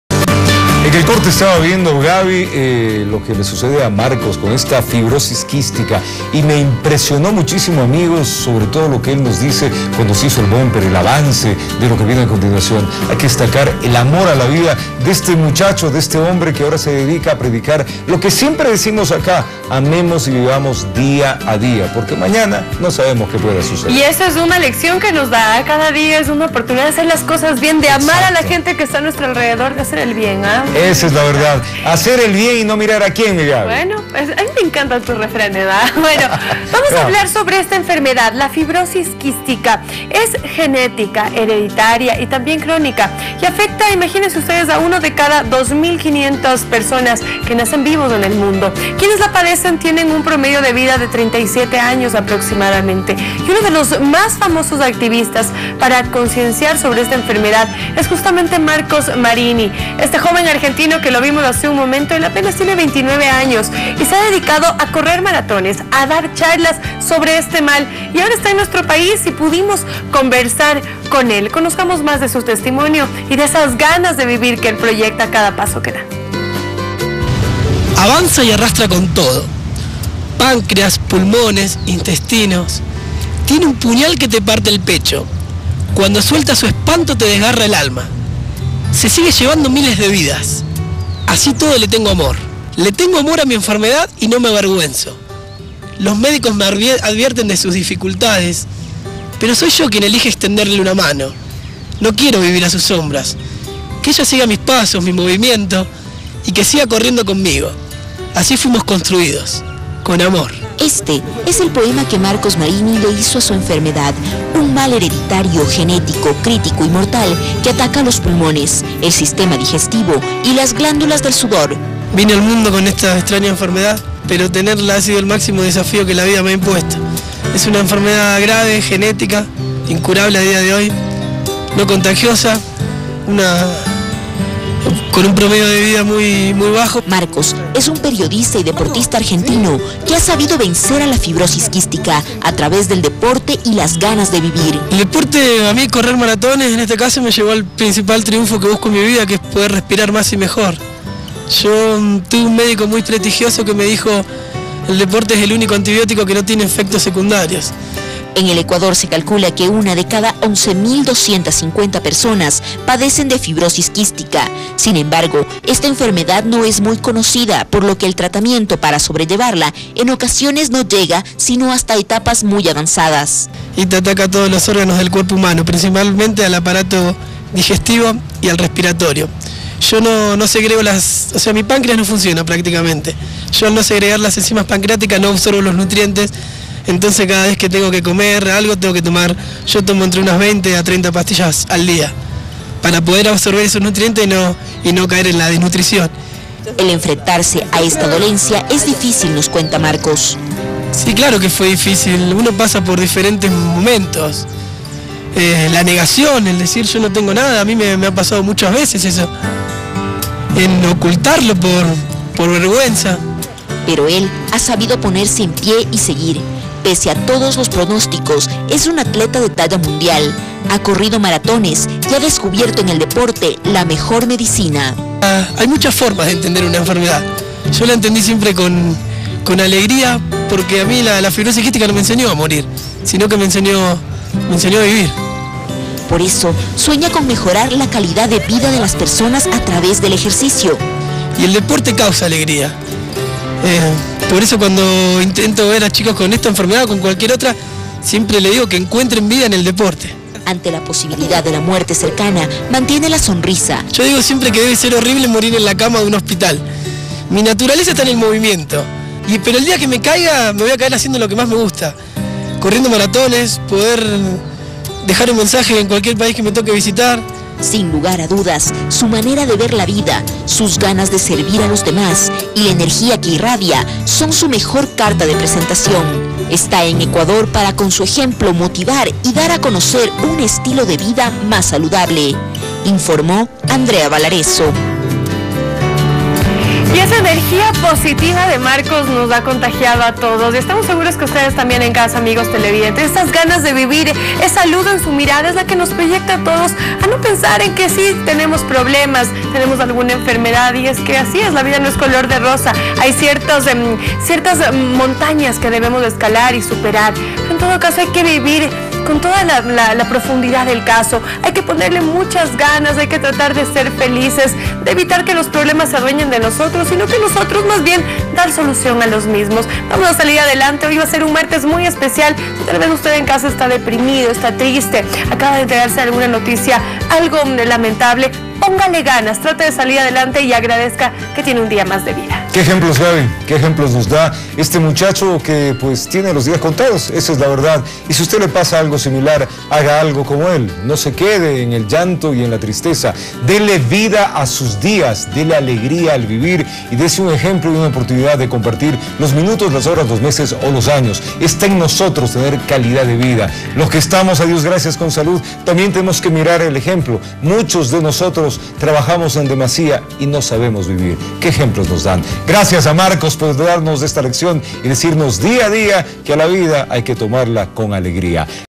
Testimonio de Marcos Marini Rivera en RTS (TV de Ecuador)
Marcos Marini Rivera en representación de la Fundación SecretoFiqui, da su testimonio en el canal RTS de Ecuador. Podrán reconocer la importancia que él transmite, su pasión y su dedicación, para que más gente con ésta enfermedad se inicie a la actividad deportiva, lema de nuestra bandera.